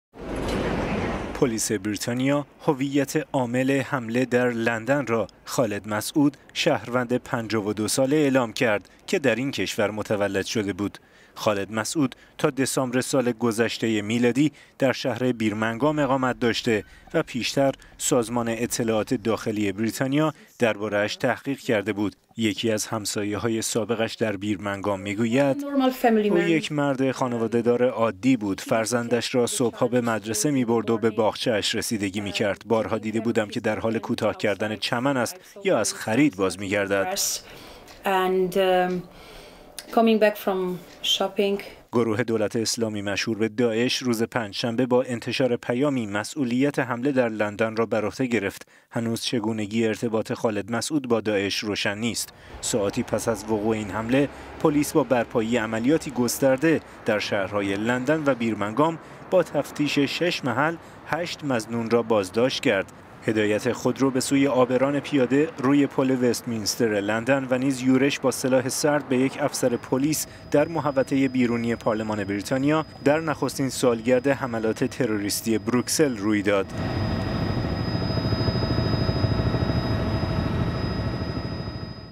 «خالد مسعود» شهروند بریتانیایی؛ عامل حمله در لندن
پلیس بریتانیا هویت عامل در حمله لندن را خالد مسعود، شهروند ۵۲ ساله اعلام کرد که در این کشور متولد شده بود.

خالد مسعود تا دسامبر سال گذشته میلادی در شهر بیرمنگام اقامت داشته و پیش تر سازمان اطلاعات داخلی بریتانیا درباره اش تحقیق کرده بود.

یکی از همسایه های سابق اش در بیرمنگام با شناسایی عکس جسدش می گوید: «او یک مرد خانواده دار عادی بود. فرزندش را صبح ها به مدرسه می برد و به باغچه اش رسیدگی می کرد. بارها دیده بودم که درحال کوتاه کردن چمن است یا از خرید بازمی گردد.»